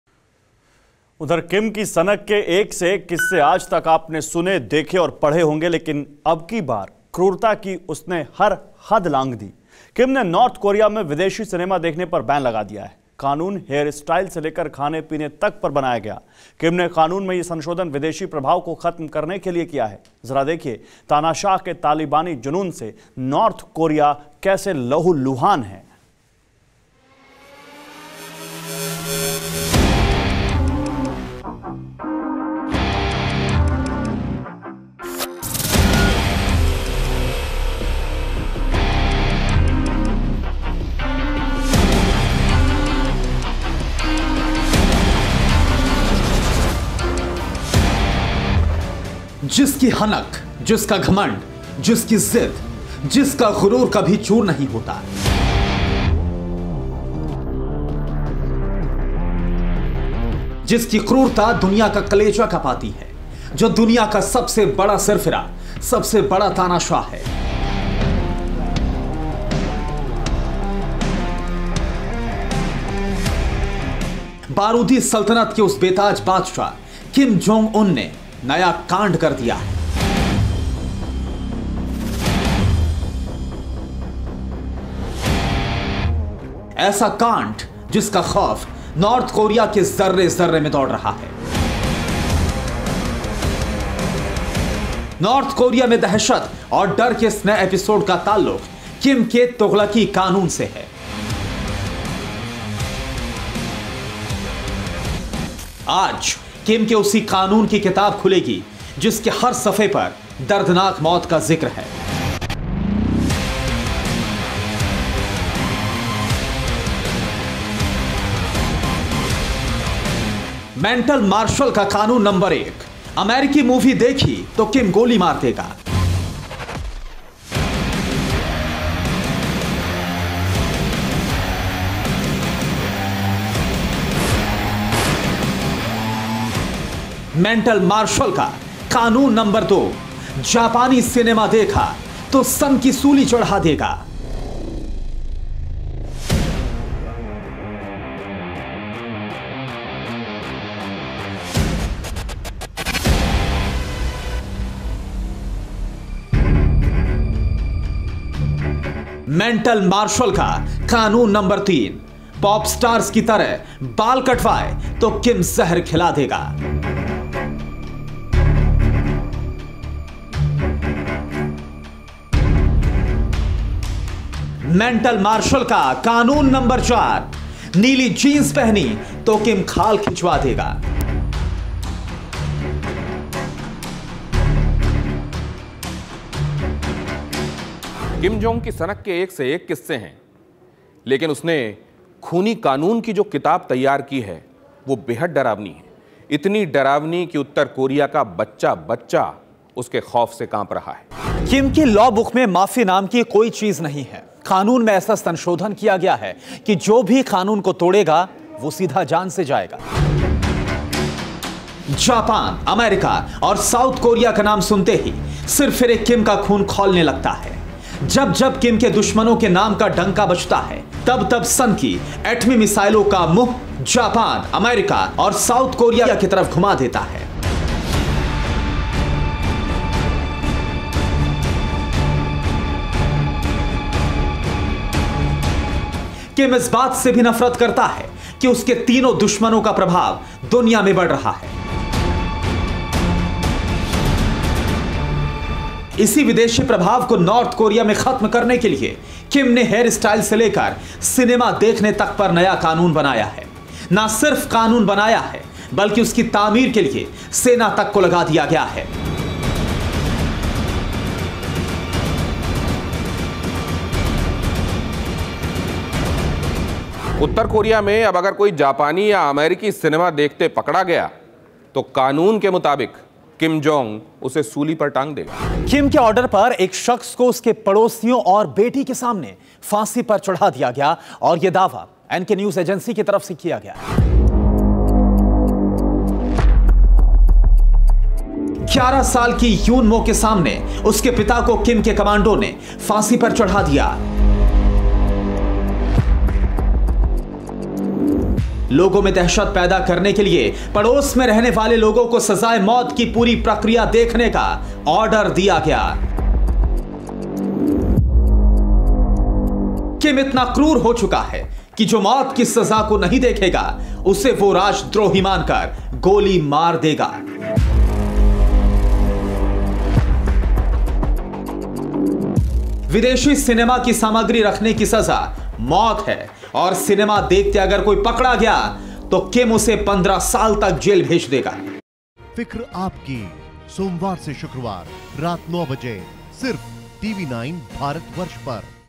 तानाशाह Kim Jong Un का तालिबानी जुनून, North Korea में हर तरफ लिखी मौत!
किम जोंग उन ने अबकी बार क्रूरता की हर हद लांघ दी.  किम ने नॉर्थ कोरिया में विदेशी सिनेमा देखने पर बैन लगा दिया है. कानून हेयर स्टाइल से लेकर खाने पीने तक पर बनाया गया. किम ने कानून में ये संशोधन विदेशी प्रभाव को खत्म करने के लिए किया है. तानाशाह के तालिबानी जुनून से नॉर्थ कोरिया कैसे लहूलुहान है.
Credit